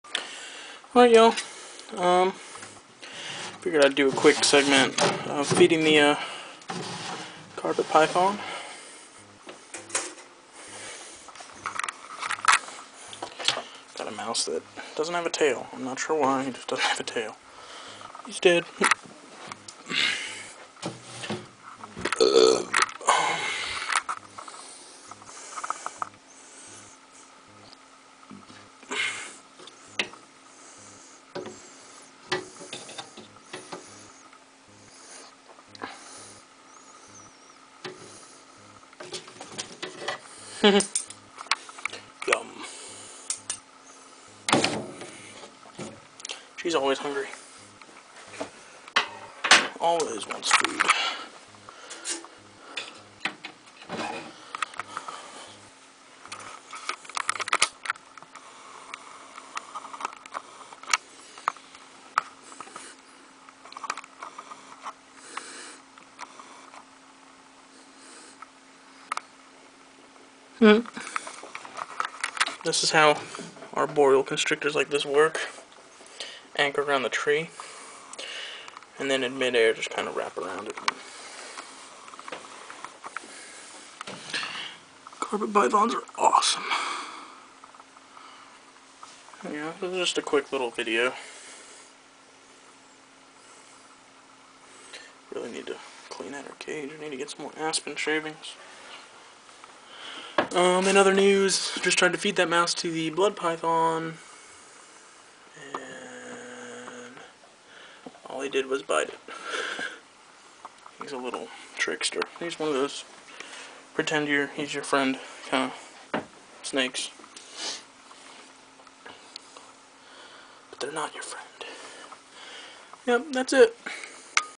Quick Carpet Feeding
just a short vid of me throwin the carpet python a snack.  hope y'all enjoy !